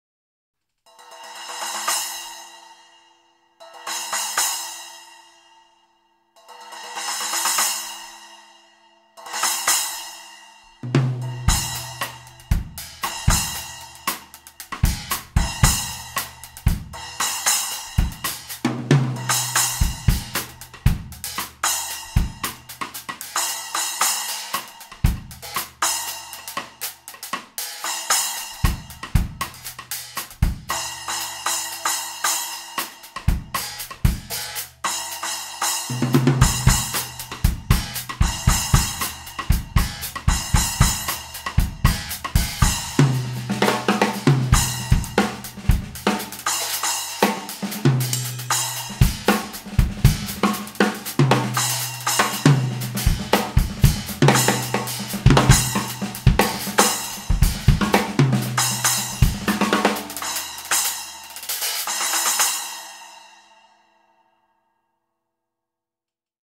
Sabian 10" XSR Splash Cymbal - Brilliant - 251g (XSR1005B-1010623L)
Sabian 10" XSR Splash Cymbal - Brilliant available from myCymbal.com

Other gear used in this video:
• Cymbals (From drummer's L to R): Sabian 14" HHX Compression Hi-Hats, Sabian 17" HHX Legacy Crash Cymbal, Sabian 20" HHX Evolution Ride Cymbal